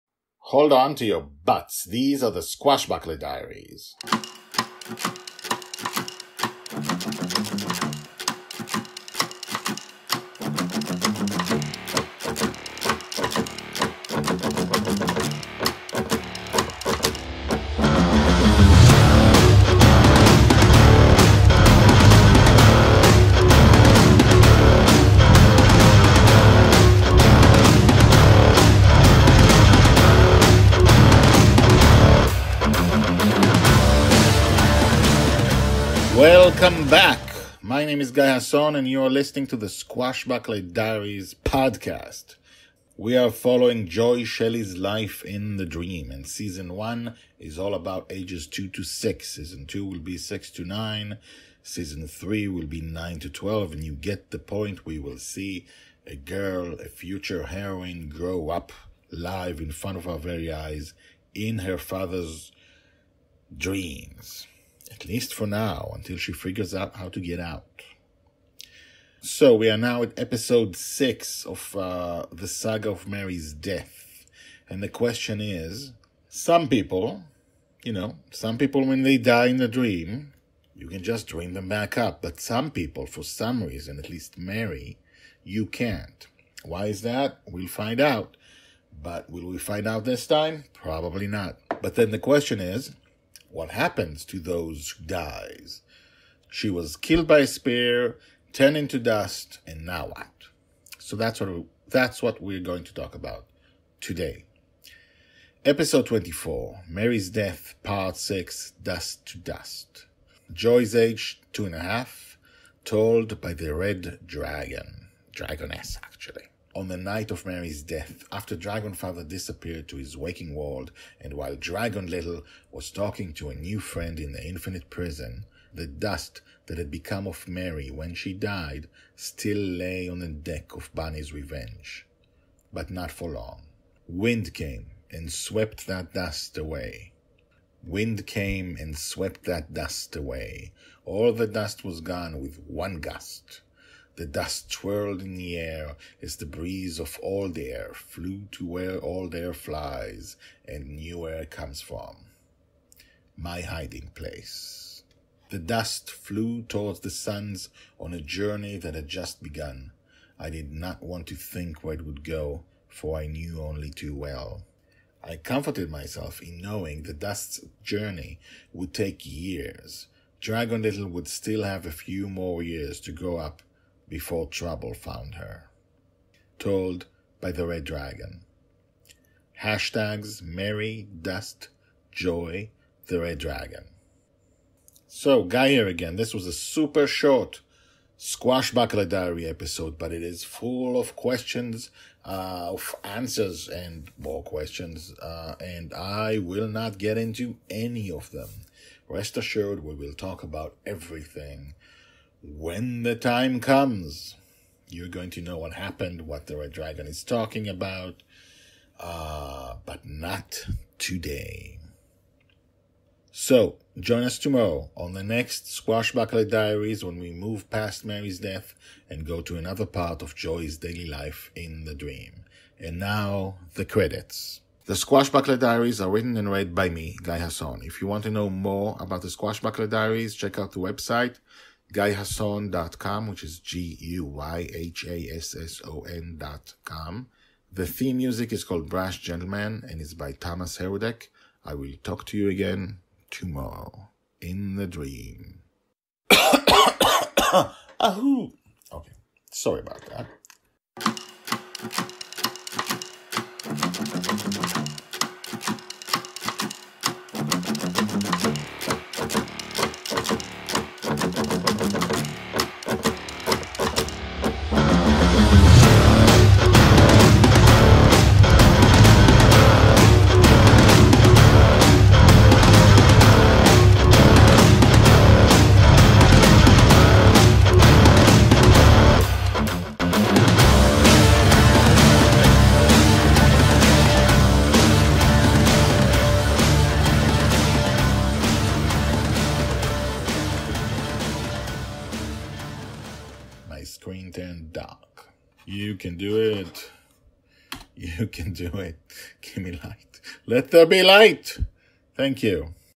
The Squashbuckler Diaries Podcast S01E24 Mary's Death, Part 6, Dust
Joy Shelley is the girl who lives in her father's dream. They live on a flying pirate ship called Bonny's Revenge. Together they have adventures and defeat villains. But when her father wakes up, she remains behind, all alone. The Red Dragon watches over her without her knowledge. She is also the one who tells the Squashbuckler Diaries about Joy, whom she calls Dragon Little.

Written and read by SF author Guy Hasson.